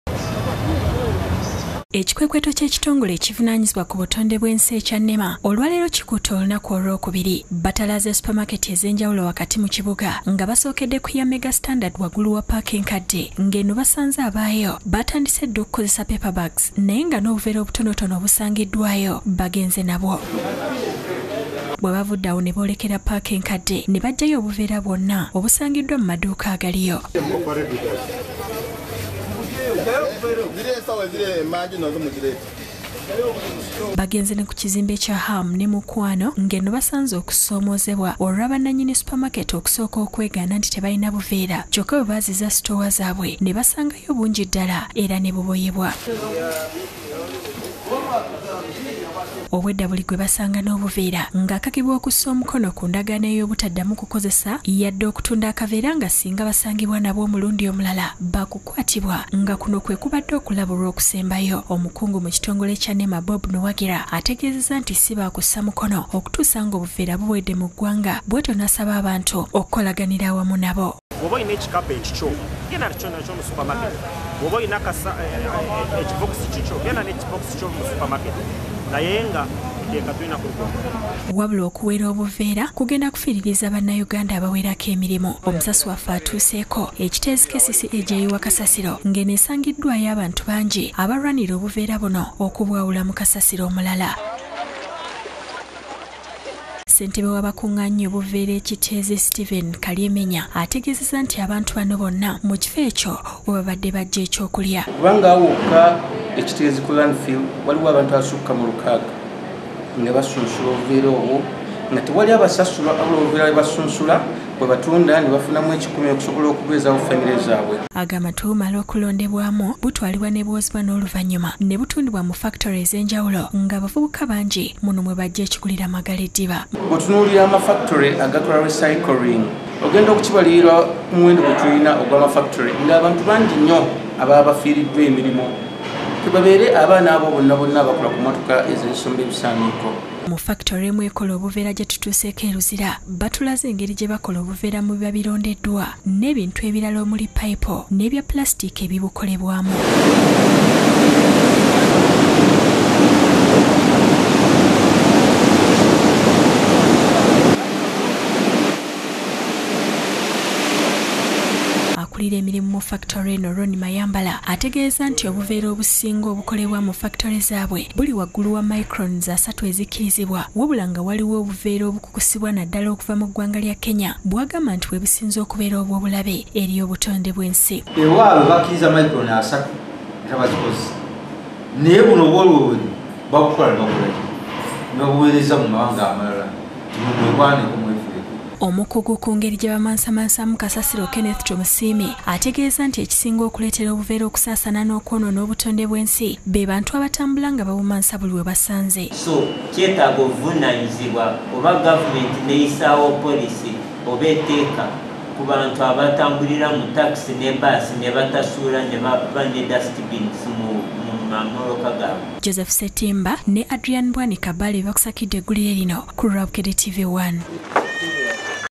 Ekikwekweto ku bakozesa obuveera
Abantu bangi boolekedde okufiirwa emirimu